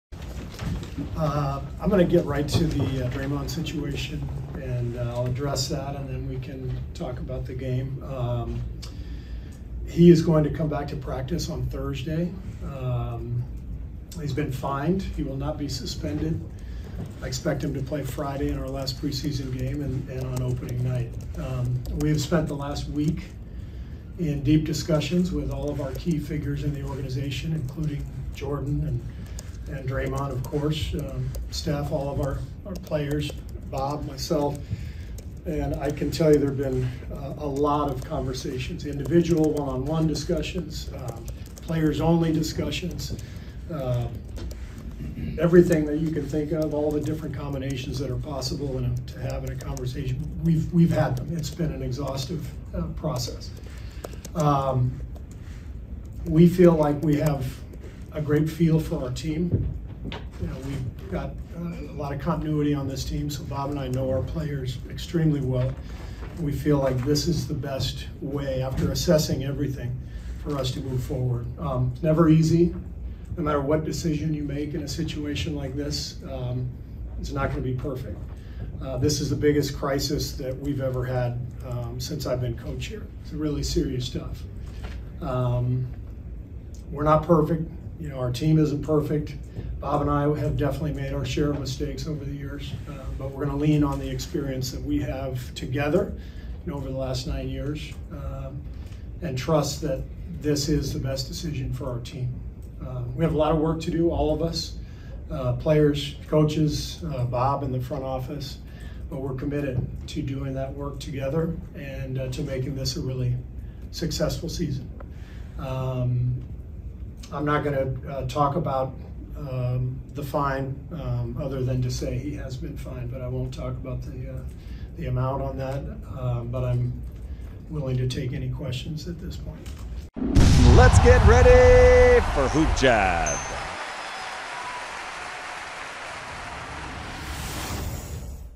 Steve Kerr Reveals Draymond Greens Punishment. Says Poole Is Ready For His Return. HoopJab NBA
Golden State Warriors Head Coach Steve Kerr reveals Draymond Green's punishment and his return date following his fight with Jordan Poole.